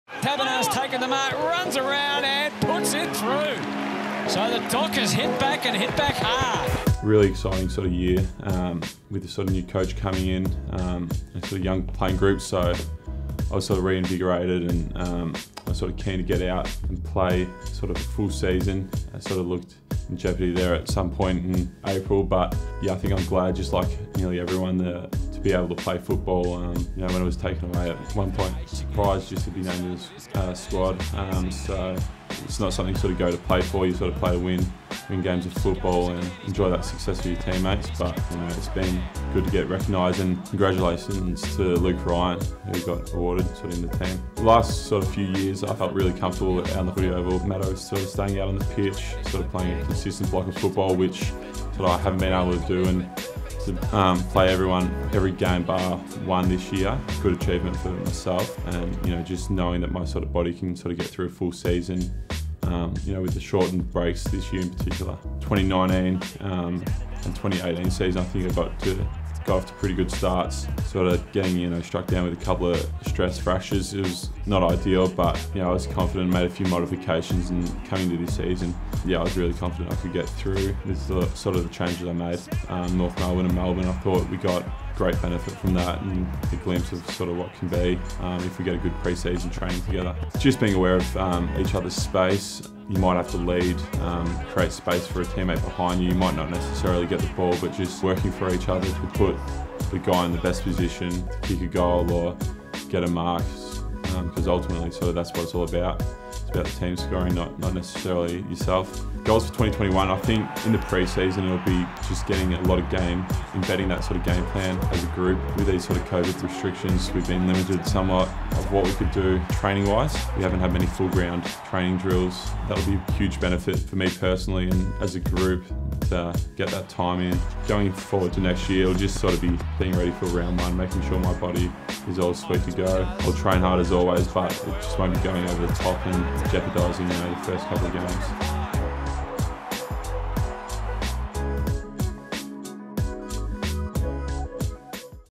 Season Review | Matt Taberner
Matt Taberner chats candidly as he looks back on his best year to date in the AFL, and the changes he has made to keep himself out on the park.